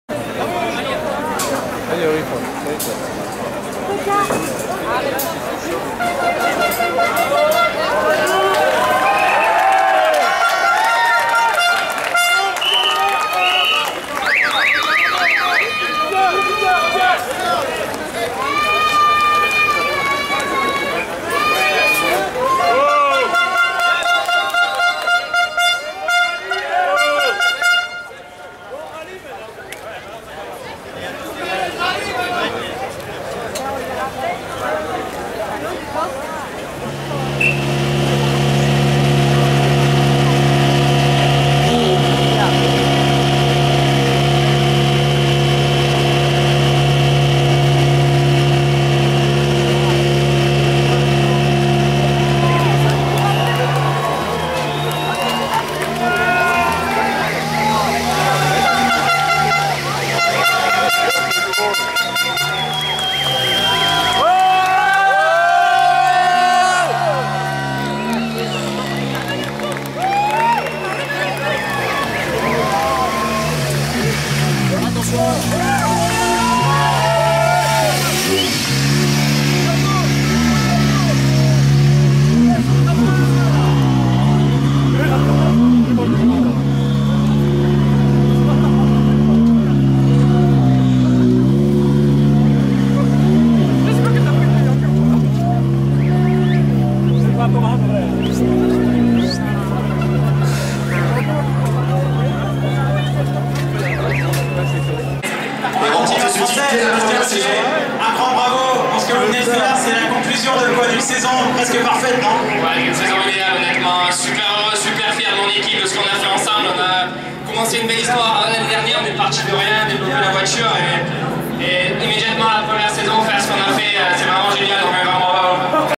S.Ogier - Rallye De France Alsace 2013 - Celebration - ESS1 Strasbourg Wacken
Bain De Foule Et Celebration Pour Sebastien Ogier Apres L'ESS1 Strasbourg Wacken Lors Du Rallye De France Alsace 2013.